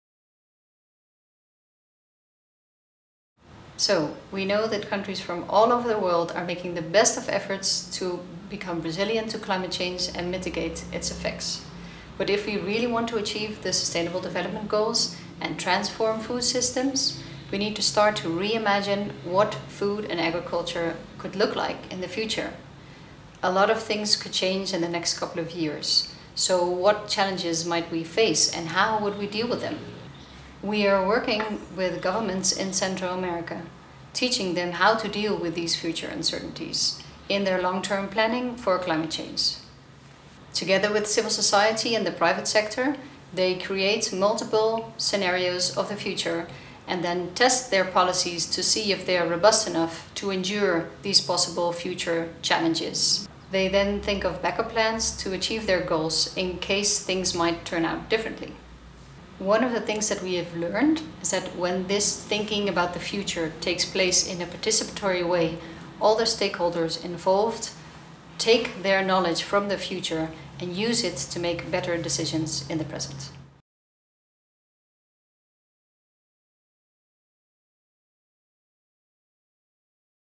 Marieke Veeger: Reimagining future food systems | NewEra4Food
Marieke Veeger, Scenarios Coordinator for Latin America at CCAFS/UCI, taking about her work with governments in Central America to help them re-imagine how to deal with future uncertainties of food and agriculture under climate change.

The video was made for the 'Transforming innovation systems to deliver impacts at scale' session at the full-day, around-the-world virtual relay event 'A new era for food and climate: Driving transformative actions'.